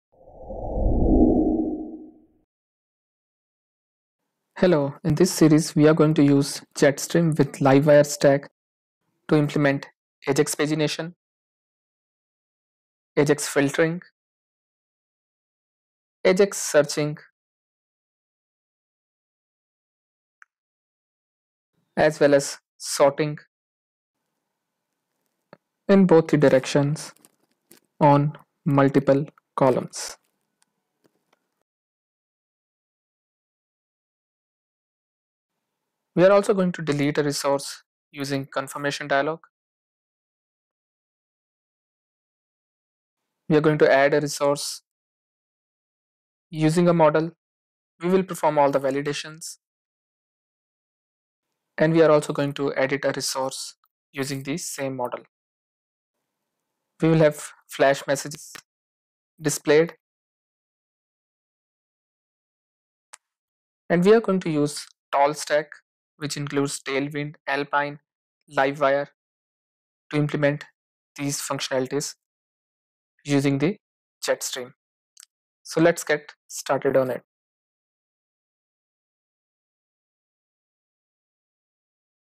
Jetstream Livewire CRUD with Tailwind Modal - Introduction
We are going to provide an overview of the functionality that we are going to implement in this Series. The functionality include AJAX Pagination, AJAX Filter, AJAX Search, AJAX Sorting, Add Modal, Edit Modal, Delete Confirmation Modal, Validation and Flash Messages using Livewire.

The functionality will be implemented using Jetstream Livewire Stack.  This is the perfect series for those who have some understanding of Livewire and want to have a greater in depth knowledge by following practical examples.

Source Code is also provided at the end of the Series.